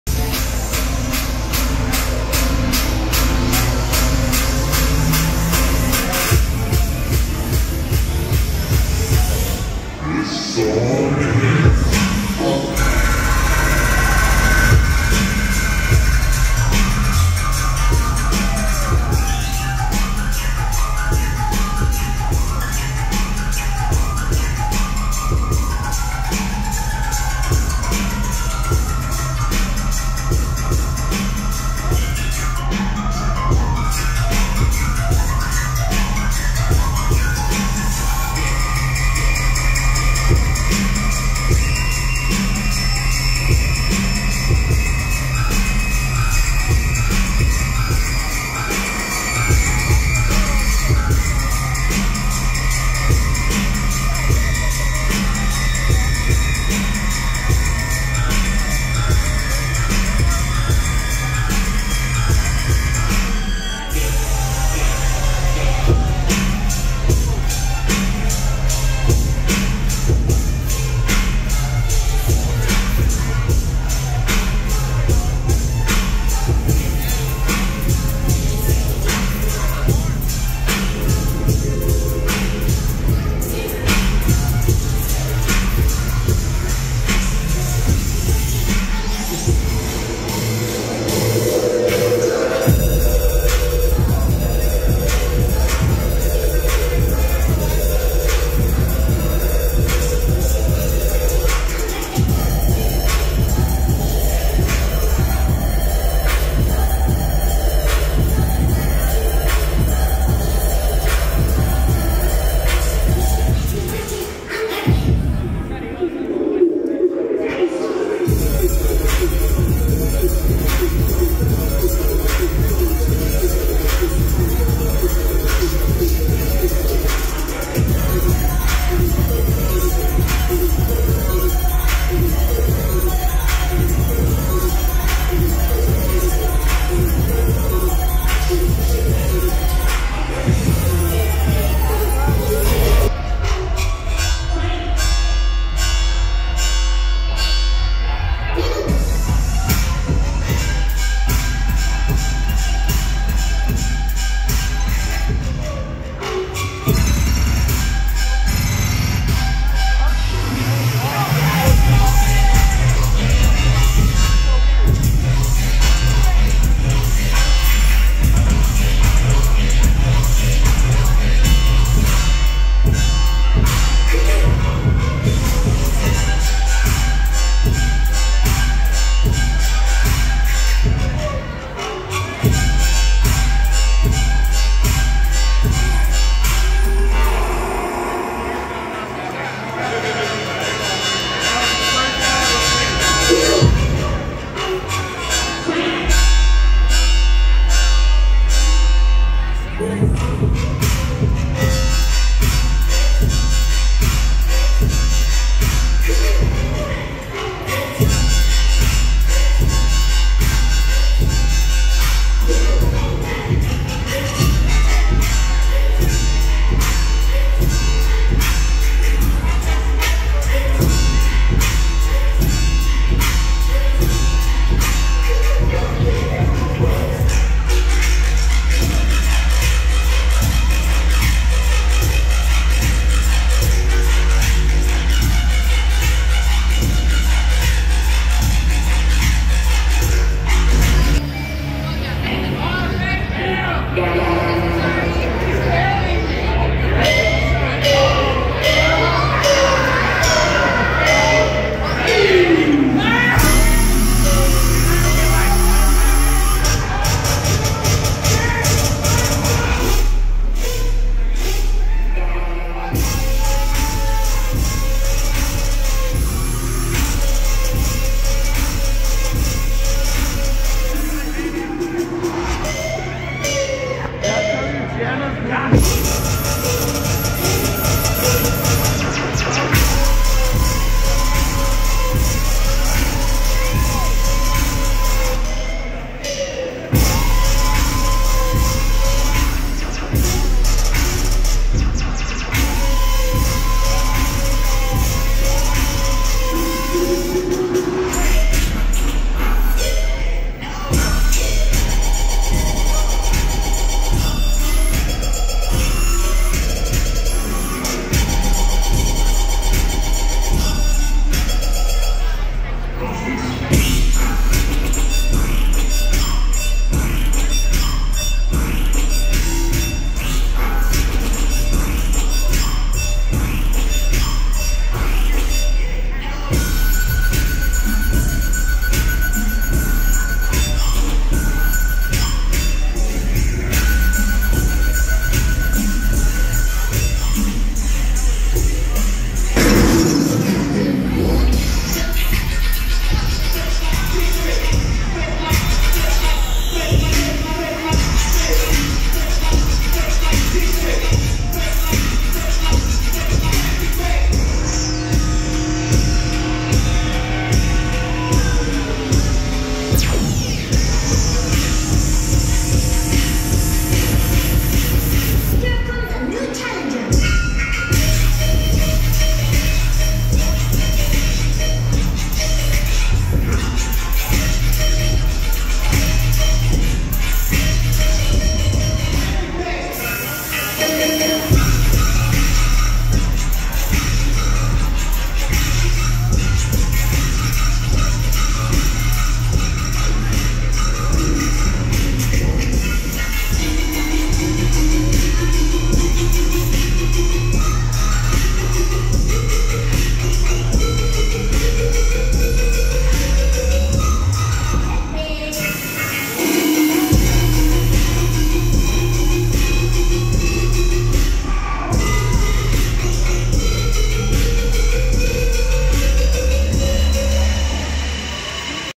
The Lost Scout @ Bass Station [360 Set]
Just pulling up to Bass Station and seeing The Lost Scout again on the decks. I wish I would've pulled up earlier but nonetheless it was cool seeing him again and seeing the whole 360 set up was something I've never seen before. Enjoy this 7 min clip of The Lost Scout on the decks.

[shot in 4K from Samsung Galaxy S22 Ultra]

[THIS FOOTAGE WAS TAKEN FOR RECREATIONAL PURPOSES ONLY. I AM NOT MAKING ANY MONEY FROM THIS VIDEO. IF YOU OWN THE CONTENT IN THE VIDEO, FEEL FREE TO MONETIZE IT.]